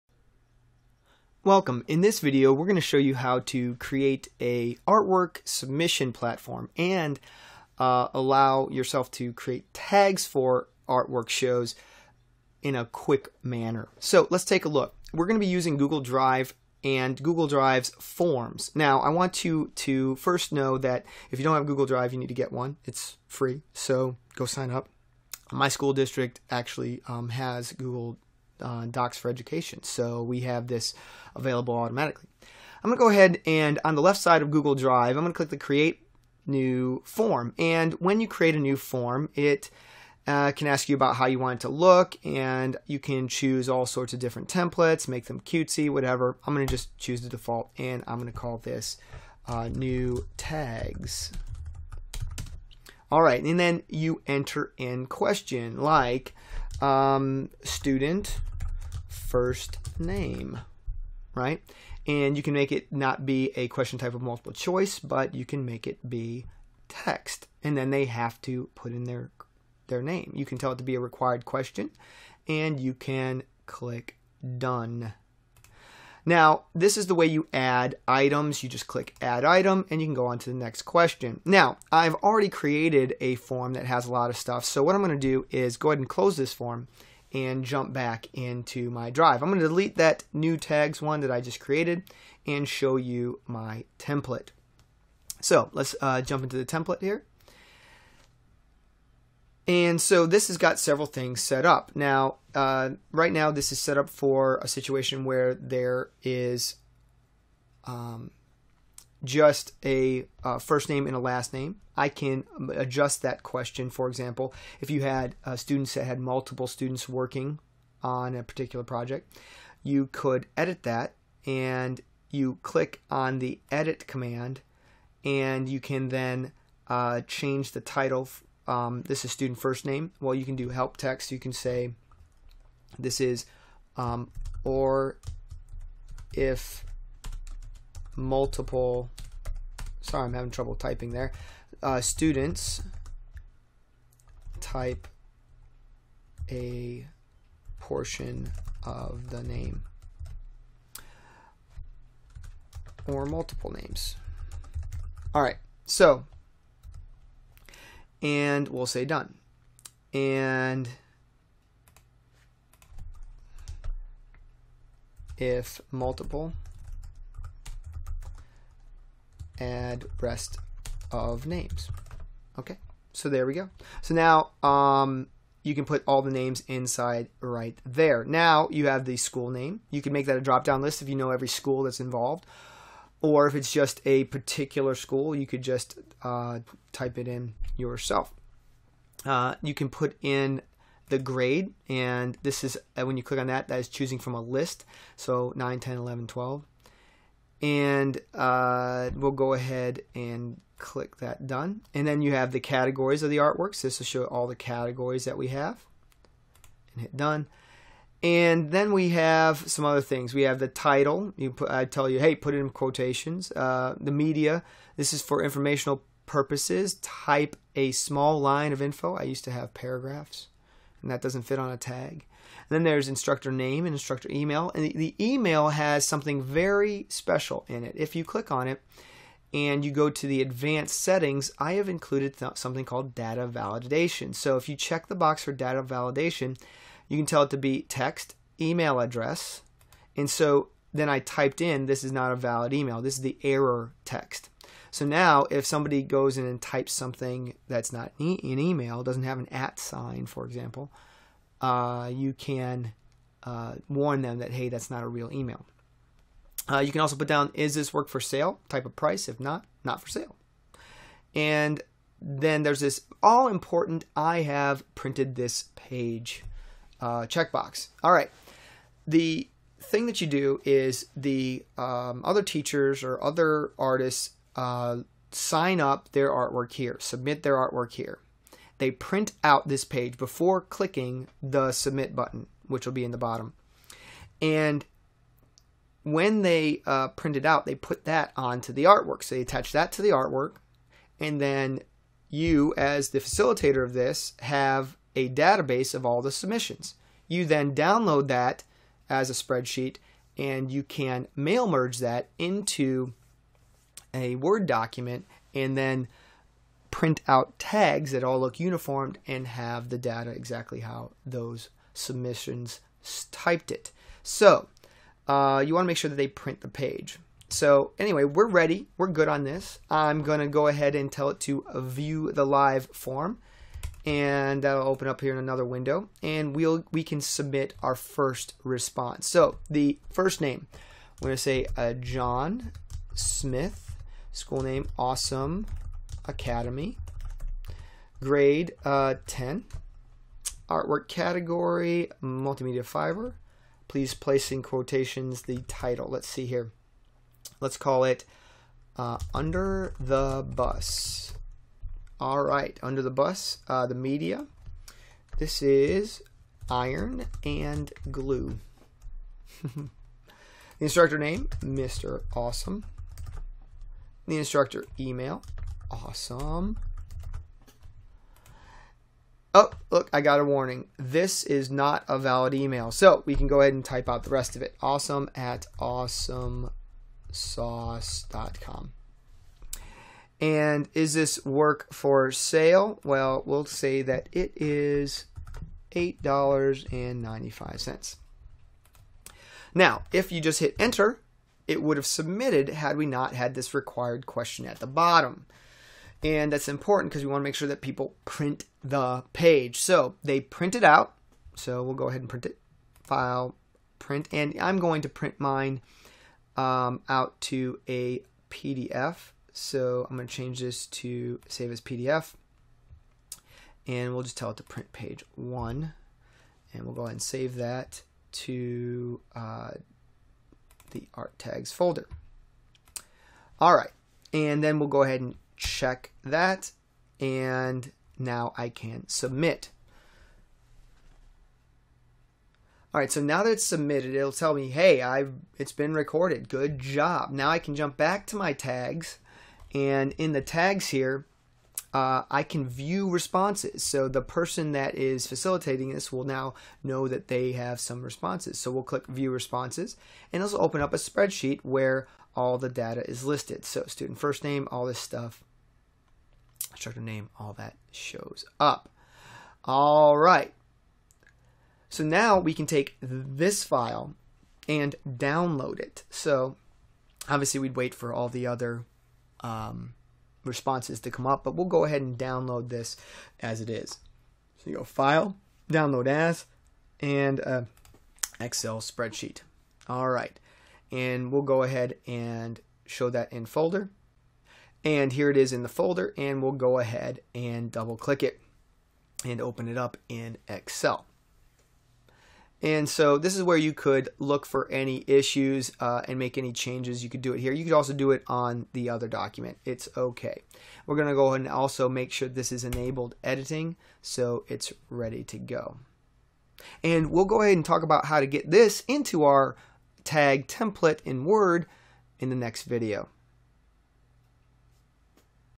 Google Apps Artwork Tags - Create Form
In this video you will learn how to create an upload form to create artwork tags for show.